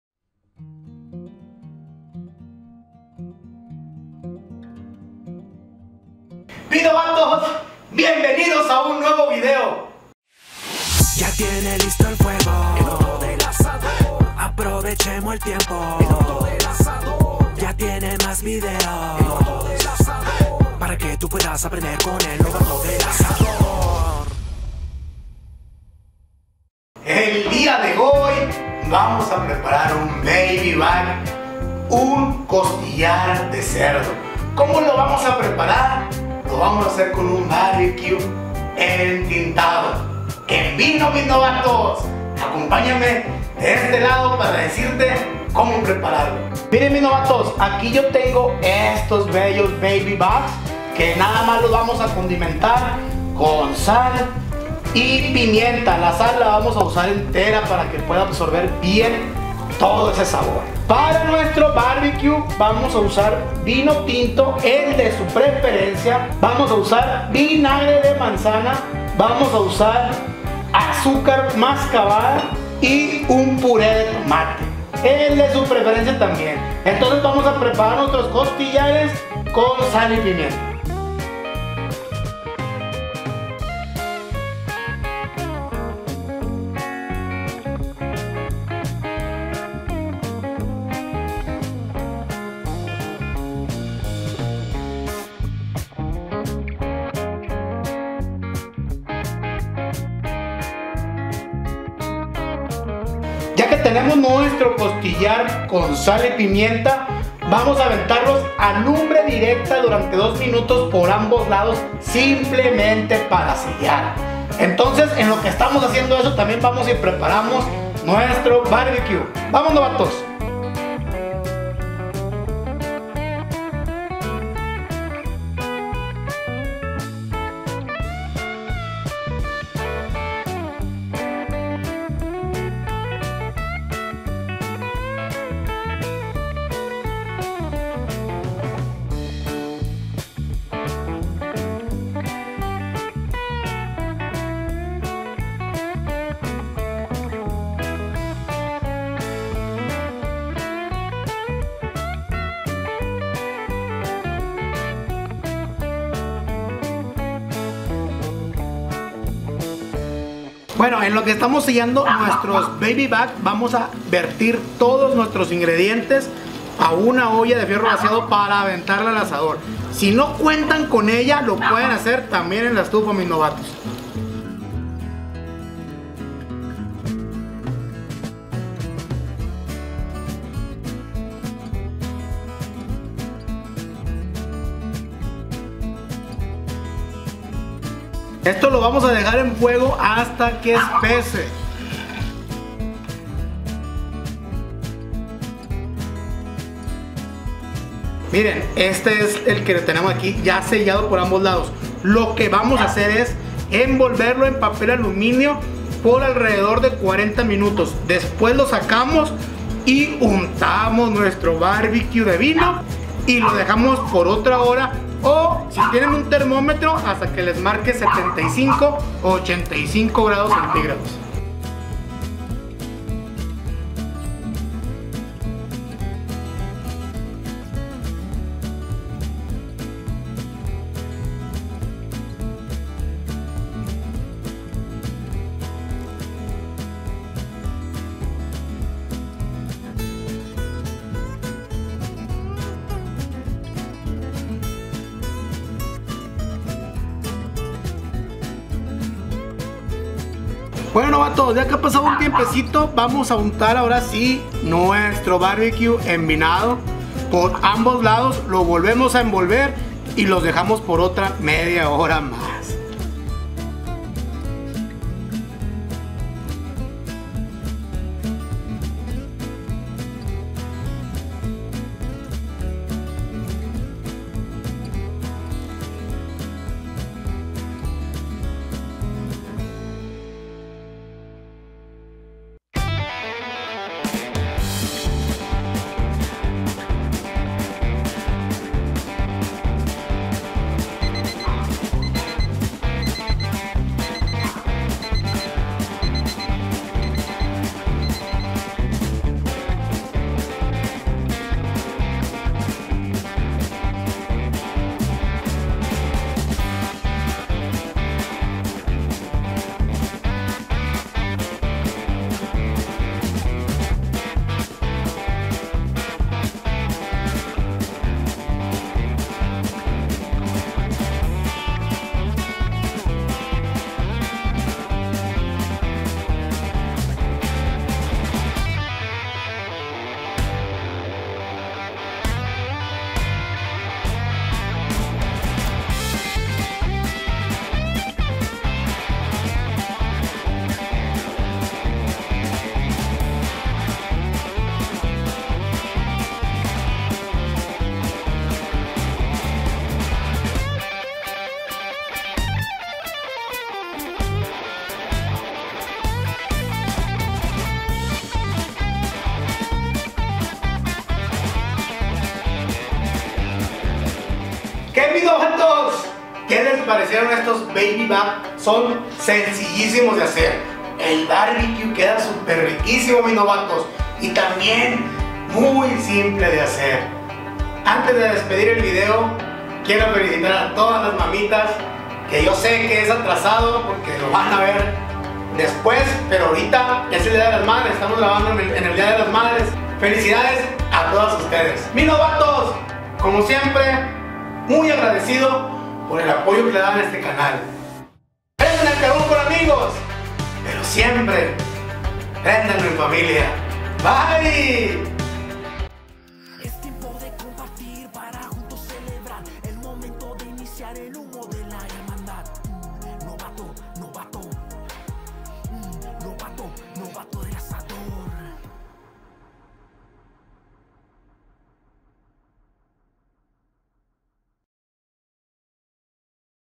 Costillas Baby back en BBQ de VINO !!!
Unas riquísimas costillas de cerdo echas en bbq de vino tinto, un manjar de los dioses.